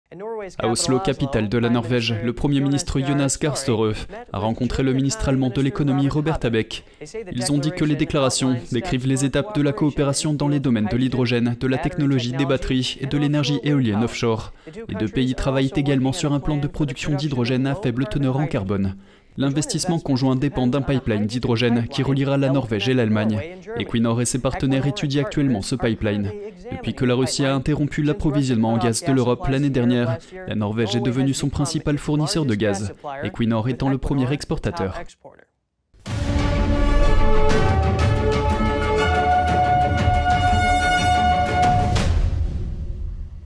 La Norvège et l'Allemagne coopèrent dans le domaine des énergies
La Norvège et l'Allemagne renforcent leur coopération dans le domaine des énergies renouvelables. Les deux alliés de l'OTAN ont signé des déclarations communes, dans le but de soulager l'Allemagne de sa dépendance au gaz russe.